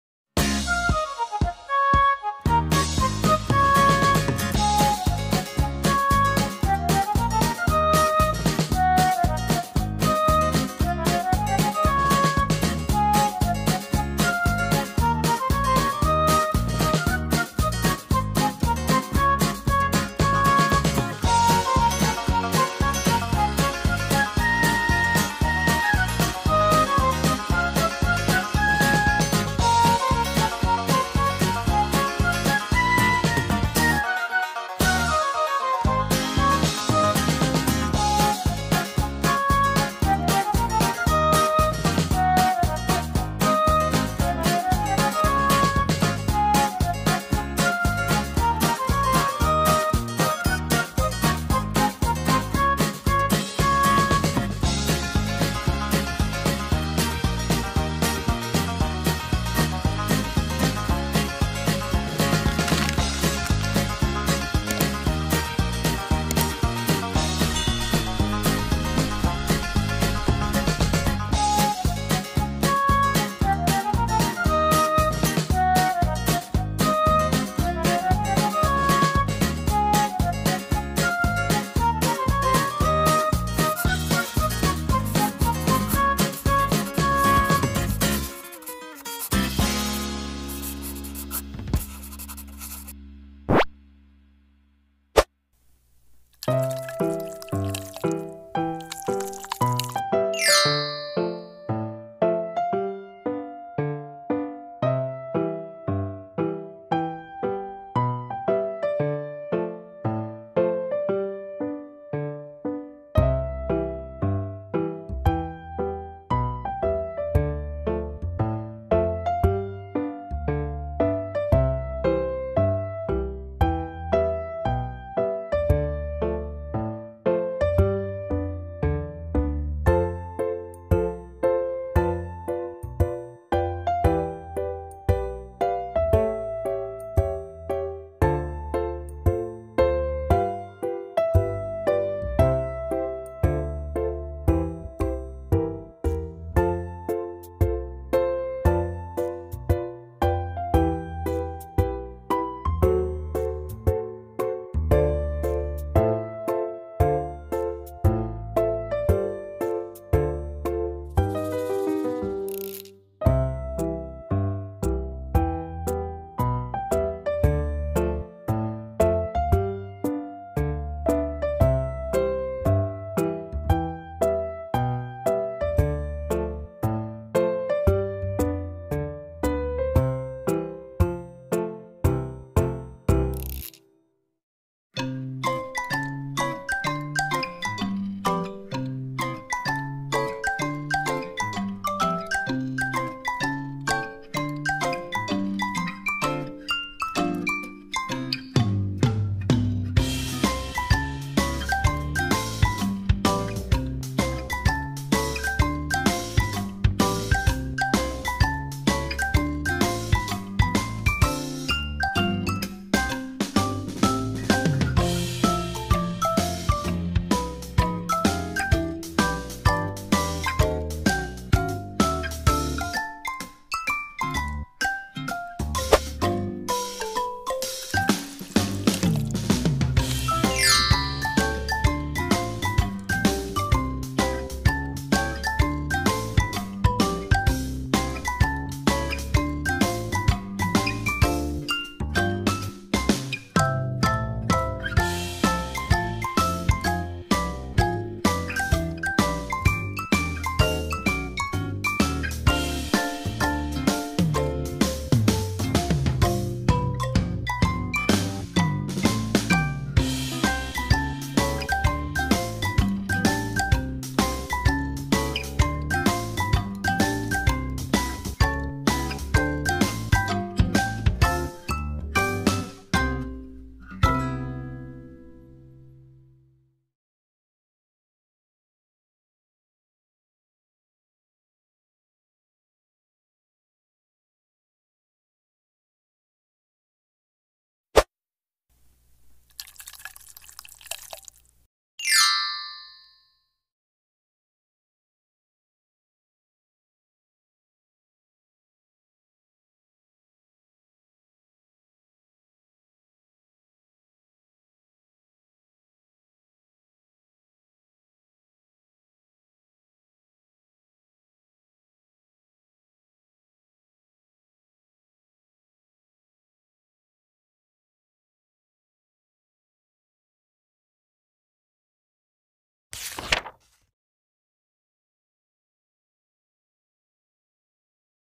How to draw a giraffe for children and toddlers | coloring & painting together
Drawing and coloring a giraffe for kindergarten & elementary school children
How to draw a giraffe cartoon
Easy giraffe drawing
Giraffe pattern
Colorful giraffe drawing